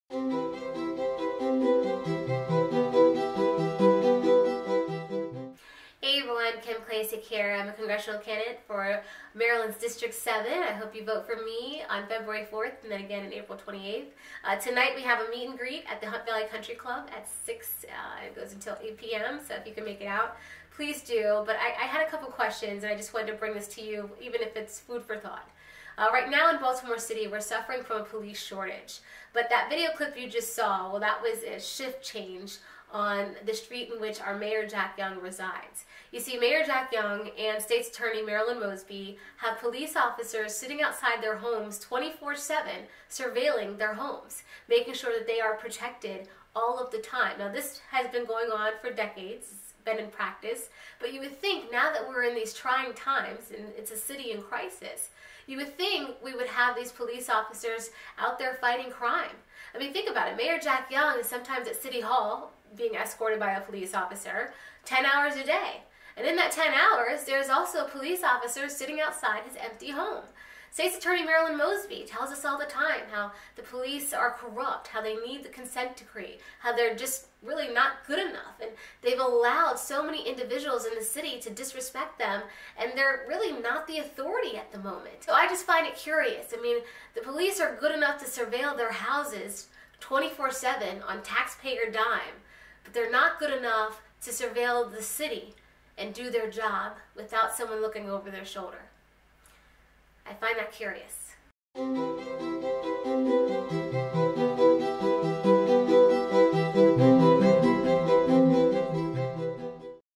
During A Police Shortage in Baltimore City Officers Are Protecting Empty Homes For Officials
KimKForCongress.com